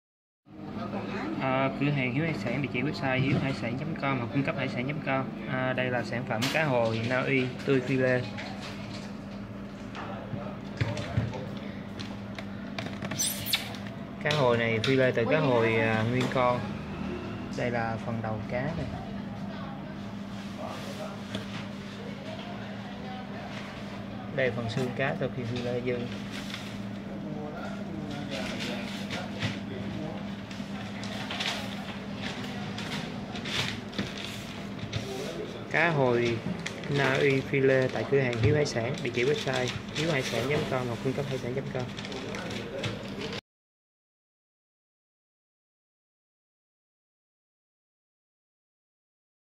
Cá hồi phi lê - Cá hồi Nauy phi lê tươi chuẩn ăn sống
Cá Hồi phi lê từ cá Hồi nguyên con nhập khẩu từ Nauy về, cá được sơ chế bỏ vẩy, xương đầu và phần mỡ. Miếng cá tươi rói có thể ăn sống hoặc chế biến nhiều món ăn khác nhau.
Quy cách: thịt cá hồi phi lê nhập khẩu tươi 100%, 1 miếng nặng từ 1 – 2,5kg.